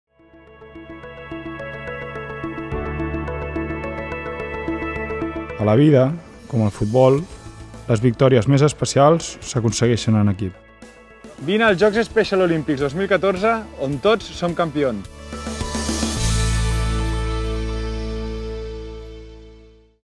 WE ARE CP | Audiovisual | Special Olympics: Gerard Piqué amb la seva cosina Monat Lacueva
Producción audiovisual con una serie de entrevistas a padres de varios atletas participantes en los Juegos Special Olympics Barcelona-Calella 2014 donde narran sus primeras vivencias al enterarse de que su hijo tenía una discapacidad. Se acompaña también de varios mensajes de deportistas famosos para promocionar el evento.